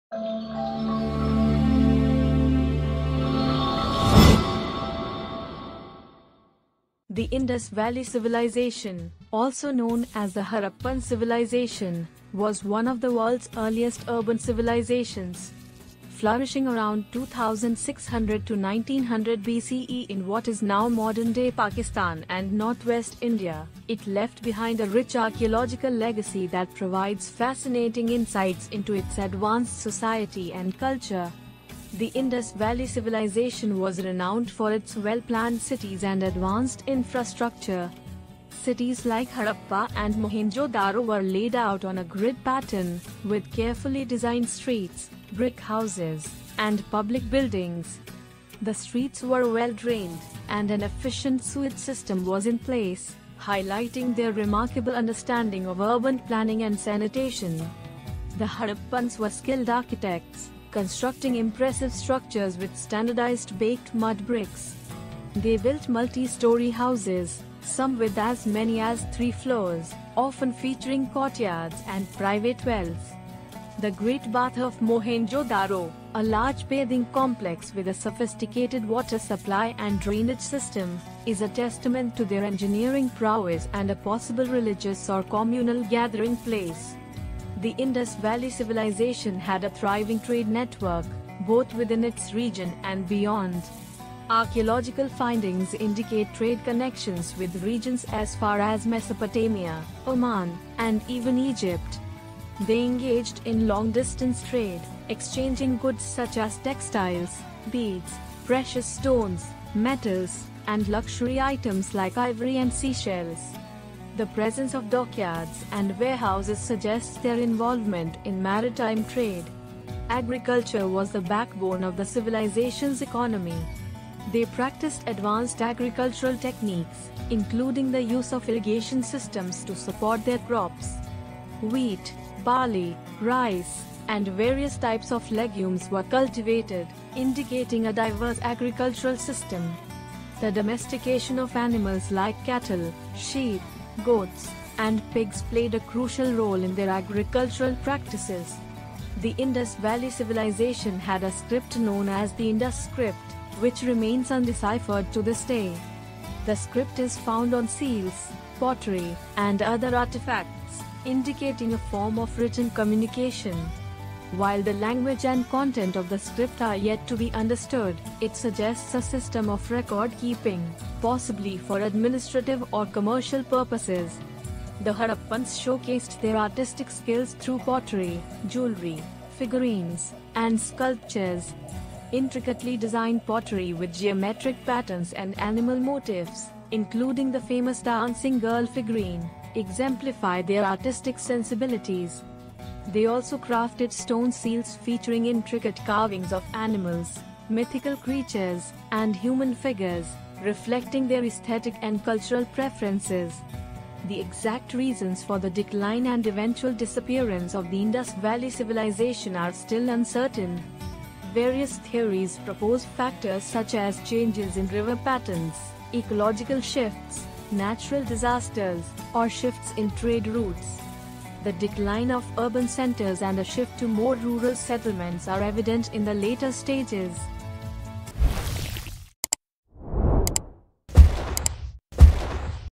Unearthing the Lost Cities: Journey into the Indus Valley Civilization
le. Join us as we explore their well-planned cities, intricate architecture, vibrant trade networks, and advanced agricultural practices. From deciphering the mysterious Indus script to unraveling their religious beliefs and artistic achievements, we embark on a captivating journey through time. Get ready to be amazed by the wonders of the Indus Valley Civilization and join the quest to unlock the mysteries of Harappa.